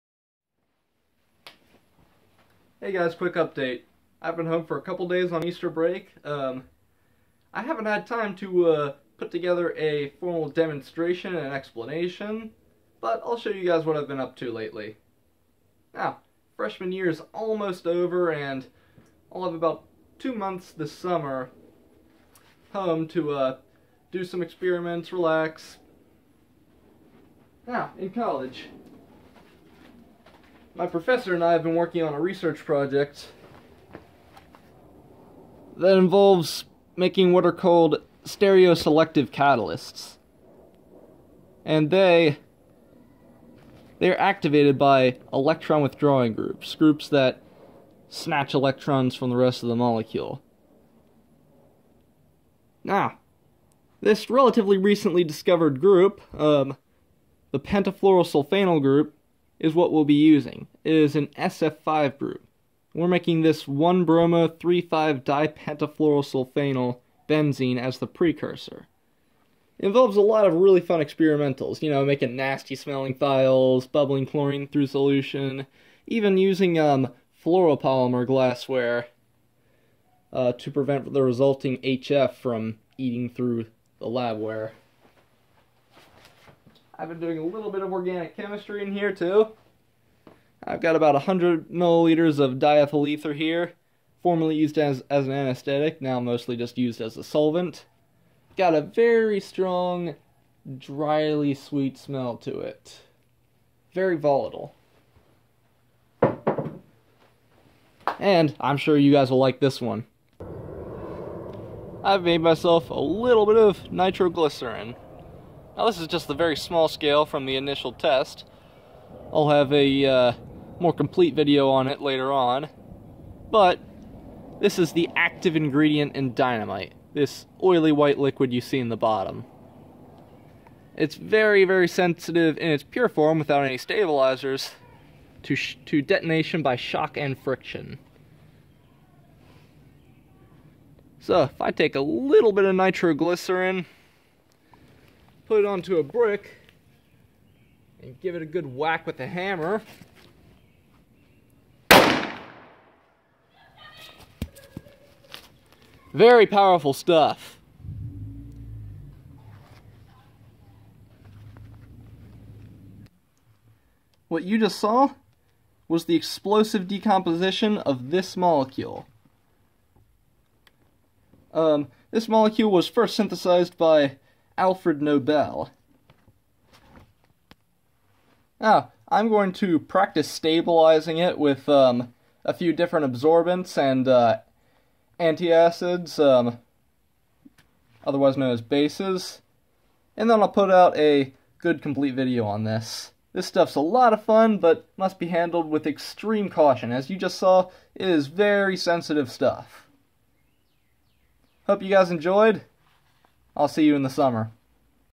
Update: Freshman year is almost over!
Quick update on what I've been up to. I'll be sure to put the link on the channel if my professor and I publish a paper on the stereoselective SF5 catalyst